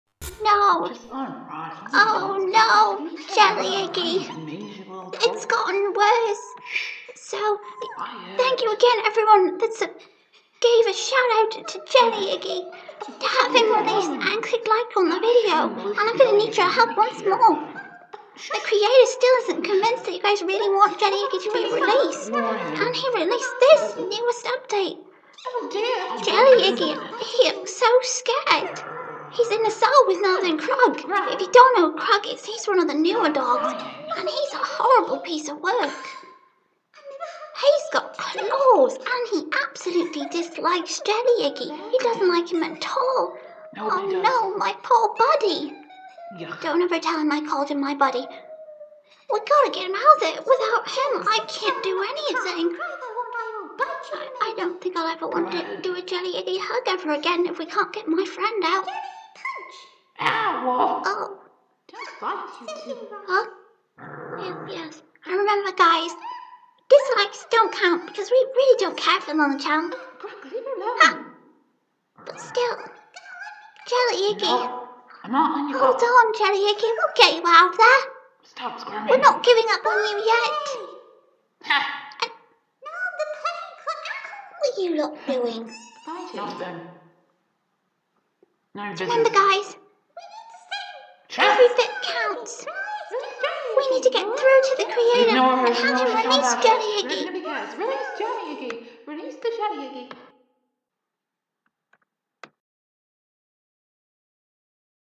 The Night Shift:Jelly Iggy News Breaking News Update - Jelly Iggy In Trouble!
Story so far - Tom the creator has kidnapped poor sweet Jelly Iggy and locket him up because apparently he is evil and also due to false chargers that the little guy apparently mugged him. Please help save Jelly Iggy we need everyones help to have him Released back to us save and sound.

In this new breaking news-  after the huge amount of support for jelly iggy with over 17 likes and 100views he still refuses to release him and has instead has released Krug on Jelly Iggy. There has been a long history with these two hating each other and that dog wouldnt care about what we want he will hurt jelly iggy.

so please help the campaign to get jelly iggy released even he doesnt deserve this.